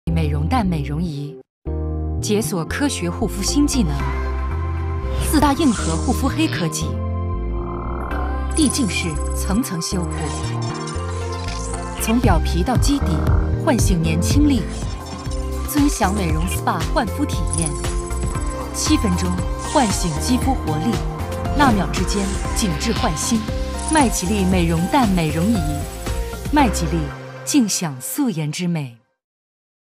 风靡护肤圈！7分钟紧致提拉平滑肌肤～麦吉丽美容蛋 Mageline Beauty Egg
麦吉丽多功能塑颜美容仪

智能新品美容仪【麦吉丽美容蛋】

纳秒之间, 肌肤紧致焕新!
紧致驻颜 | 嫩肤淡纹 | 舒缓浮胀 | 挥別泡泡脸

提拉紧致轮廓, 告别垮脸，提拉下颚线
纤紧苹果肌, 立现小V脸
全面紧致淡纹, 肌肤嘭弹细腻光滑
焕活肌底层, 促进胶原蛋白新生, 肌肤更显年轻
强化精华吸收, 修复更深入持久
破译时光肌秘, 重焕年轻光采.

这款护肤黑科技:
• 一秒上手，随时随地奢享美容院线级紧致Spa护理
• 通过四大核心美肤功能, 纳秒级脉冲震动，集束射频修护力直达肌底层
• 定制化分区护理，精准护肤无死角
• 按摩触头温和亲肤，敏感脆弱肌也可放心使用
• 只需7分钟，在家也能享受美容院级Spa焕肤体验，紧致少女肌立现

Mageline Multi-Functional Beauty Instrument --- Mageline “Beauty Egg’ Instrument, Firms and Rejuvenates Skin in NanoSeconds. With the four major black technologies of pulse vibration, nanosecond pulse, eddy current and cluster RF, it provides skin a luxurious spa-level deep care experience at home. The nanosecond-level treatment that progressively repairs layer by layer, awakens skin vitality in 7 mins, rejuvenates, lifts and firms skin, accurately reduces fine lines and wrinkles, relieves puffiness, promotes the production of collagen, rejuvenates youthful skin and makes skin plump and elastic.